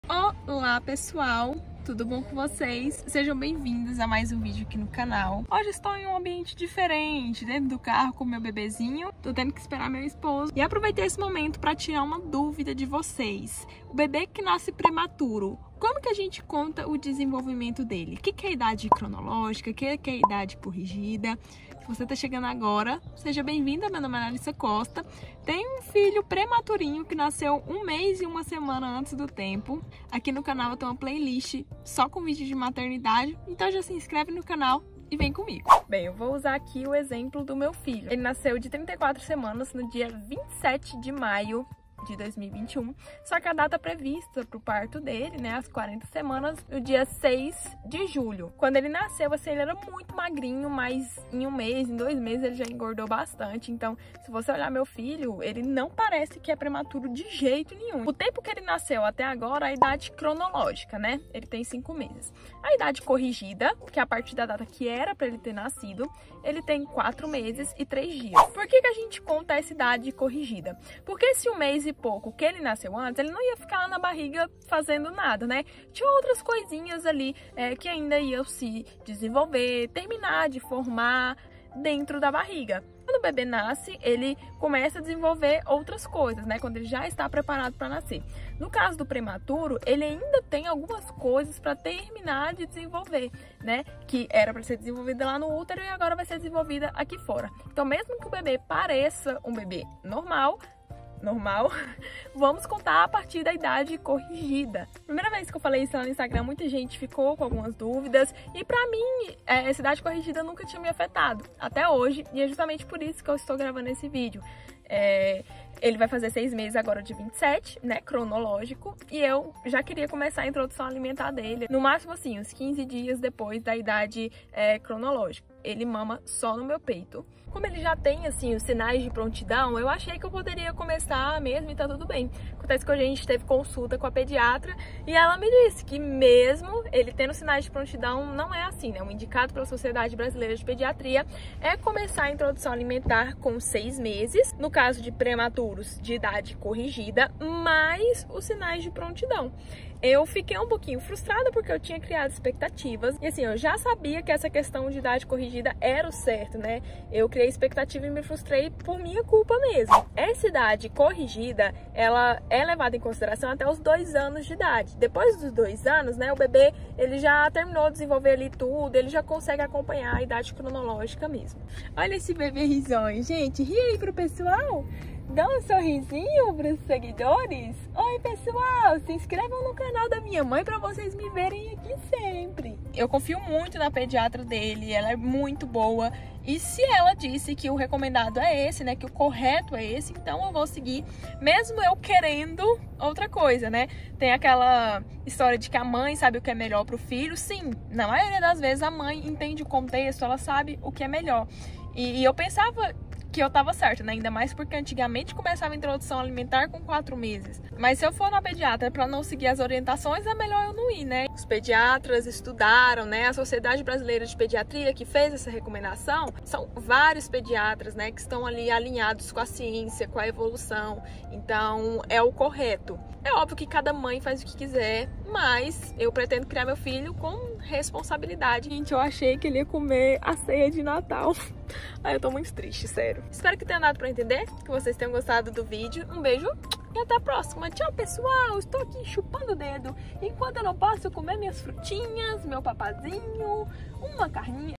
Bebê Prematuro: Idade Cronológica x Idade Corrigida
Você sabia que o desenvolvimento de bebês prematuros é contado de forma diferente de bebês que nascem a termo?

Nesse vídeo explico o que é idade cronológica e idade corrigida, até quando é contada essa diferença e por que é preciso contar dessa  maneira.

Meu bebê nasceu prematuro de 34 semanas, por isso a pediatra conta o desenvolvimento dele pela idade corrigida.